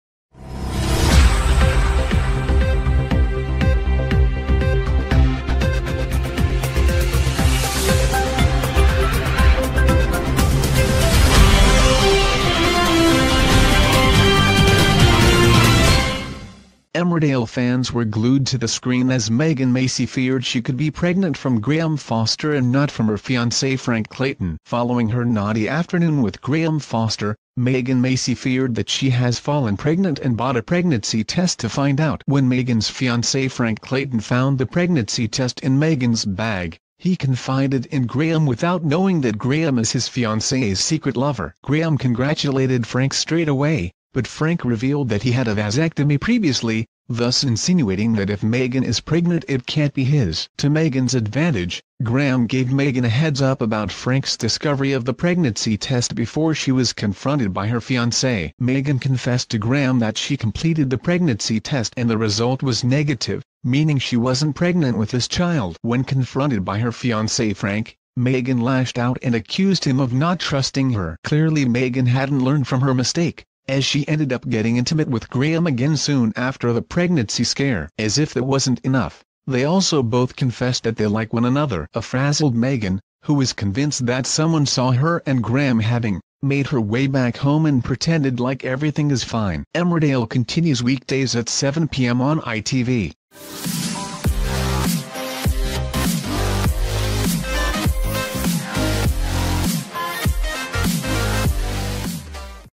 Emmerdale spoilers: Frank Clayton tipped to expose cheating fiancée Megan Macey and Graham Foster
Emmerdale spoilers: Frank Clayton tipped to expose cheating fiancée Megan Macey and Graham Foster after pregnancy BOMBSHELL
Emmerdale fans were glued to the screen as Megan Macey feared she could be pregnant from Graham Foster and not from her fiancée Frank Clayton.
Following her naughty afternoon with Graham Foster, Mega